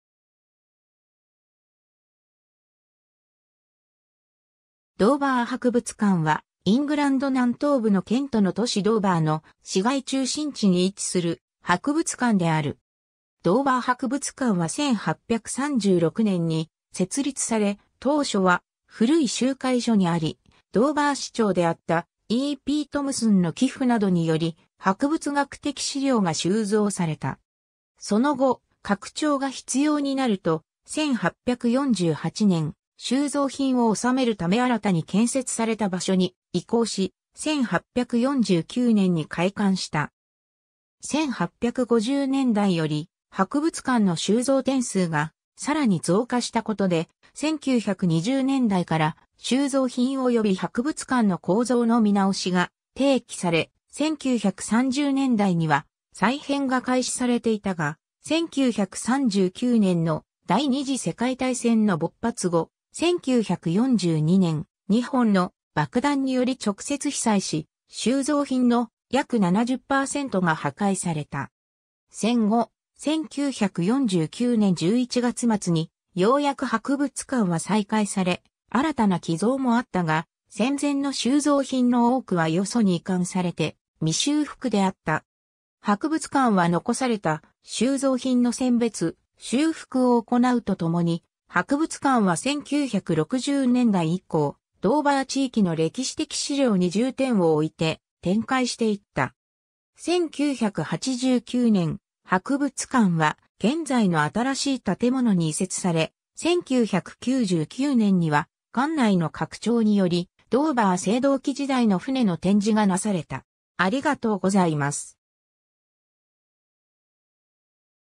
ドーバー博物館
ドーバー博物館（ドーバーはくぶつかん、英: Dover Museum）は、イングランド南東部のケント（州）の都市ドーバーの市街中心地に位置する博物館である。
 ドーバー博物館は1836年に設立され、当初は古い集会所にあり、ドーバー市長であった E・P・トムスン (E. P. Thompson) の寄附などにより博物学的資料が収蔵された。
その後、拡張が必要になると、1848年、収蔵品を収めるため新たに建設された場所に移行し、1849年に開館した。
1850年代より博物館の収蔵点数がさらに増加したことで、1920年代から収蔵品および博物館の構造の見直しが提起され、1930年代には再編が開始されていたが、1939年の第二次世界大戦の勃発後、1942年、2本の爆弾により直接被災し、収蔵品の約70パーセントが破壊された。
 戦後、1949年11月末にようやく博物館は再開され、新たな寄贈もあったが、戦前の収蔵品の多くは他所に移管されて未修復であった。
博物館は残された収蔵品の選別・修復をおこなうとともに、博物館は1960年代以降、ドーバー地域の歴史的史料に重点を置いて展開していった。
1989年、博物館は現在の新しい建物に移設され、1999年には、館内の拡張によりドーバー青銅器時代の船の展示がなされた。